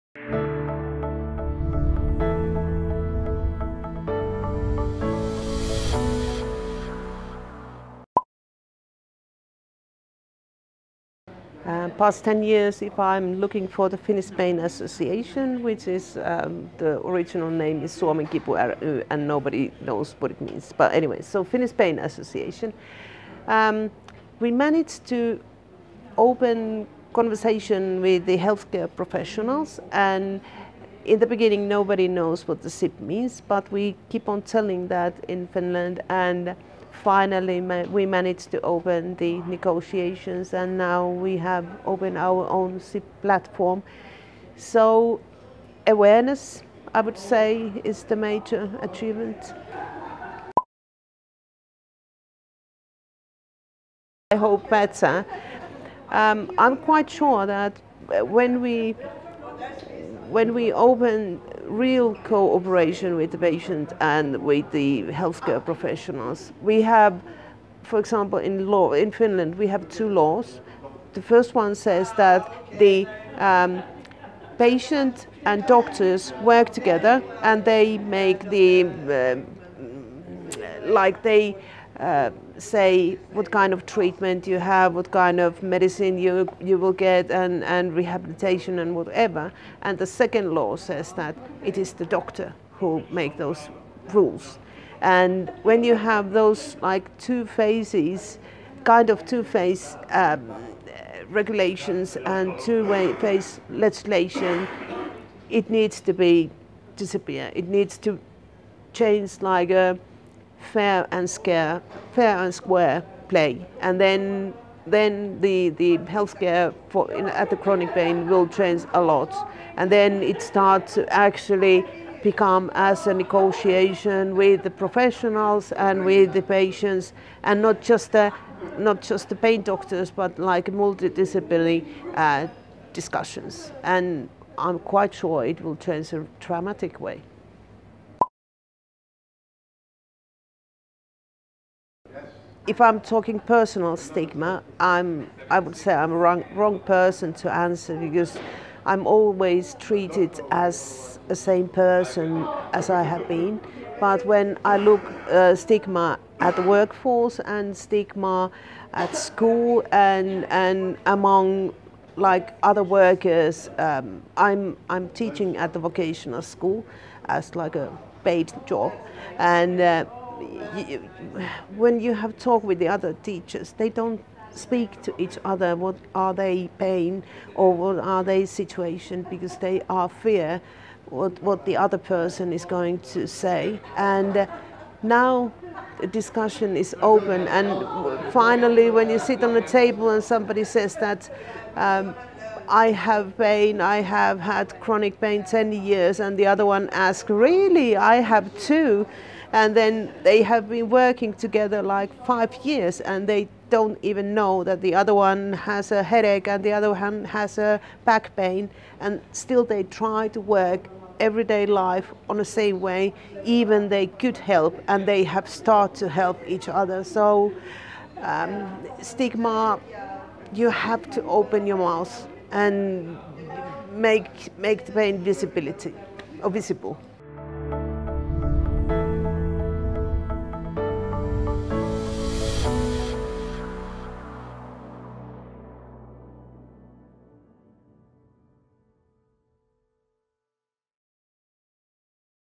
Liisa Jutila - Finnish Pain Association
Opening a local SIP platform in Finland has been a big success for visibility and awareness about chronic pain.

Liisa Jutila from the Finnish Pain Association explains how patients and doctors can work together, and the importance of encouraging patients to talk about chronic pain to make it a visible disability.